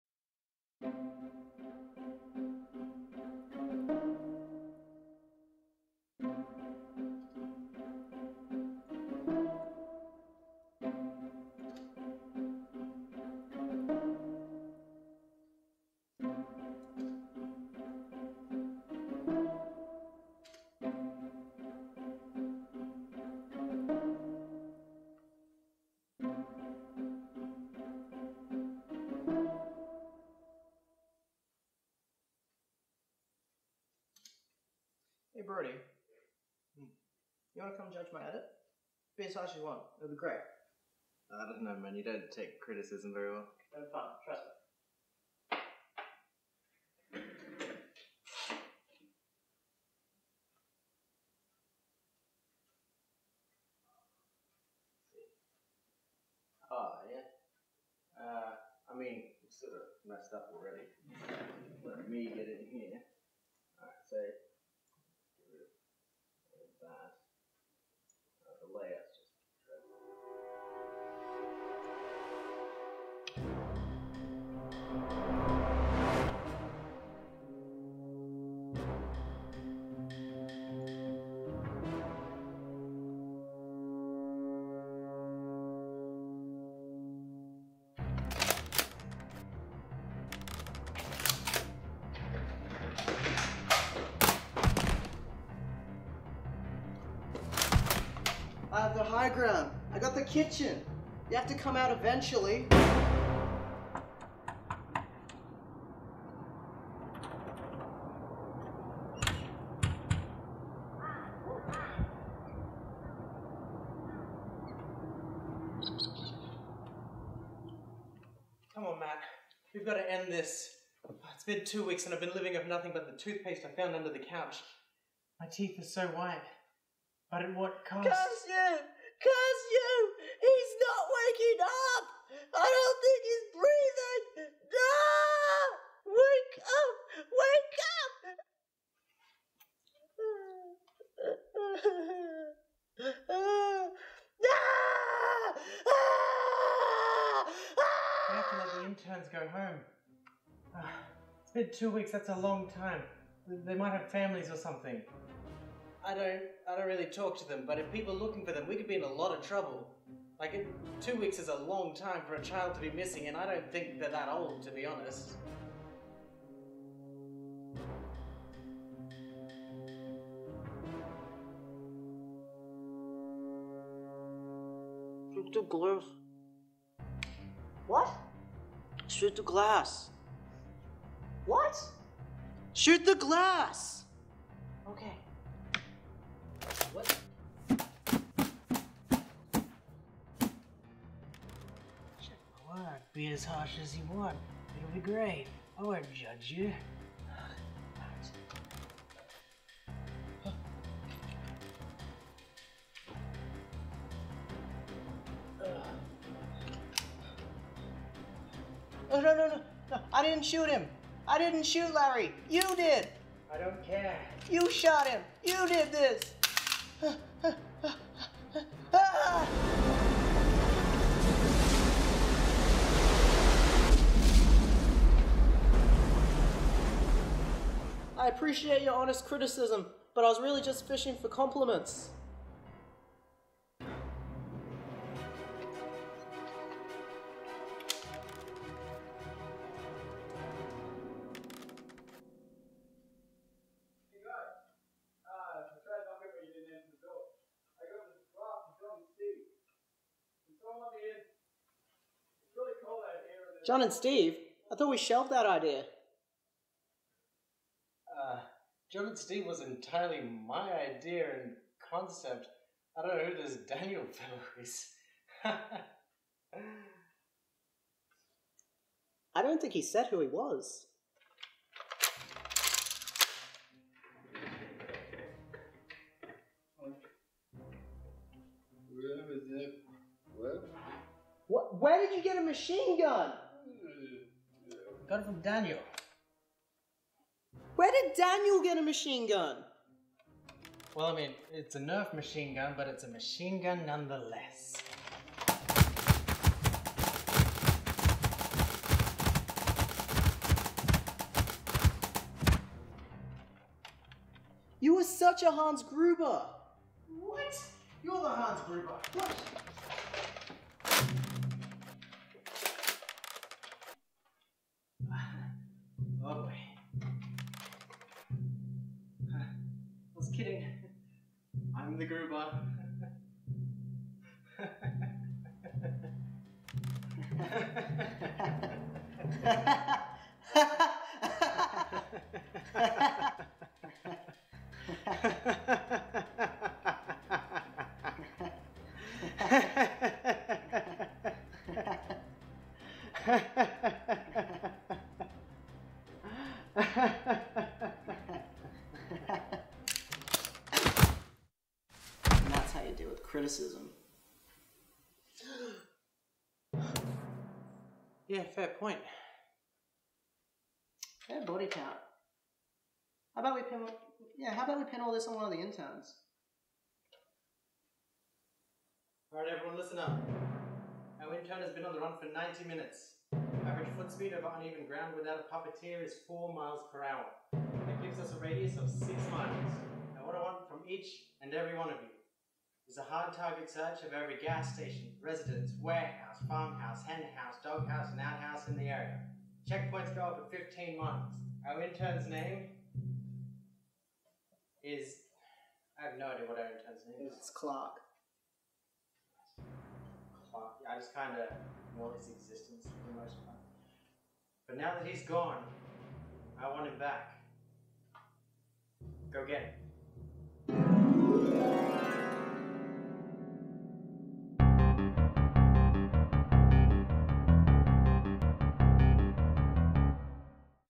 NERF HARD | Short Comedy Sketch [4K]
It's Die Hard but Nerf, kind of!
This week at Miccajet Studios we show you how to deal with critics.
Presented in 4K resolution.

Show your support and Like, Favorite and SHARE!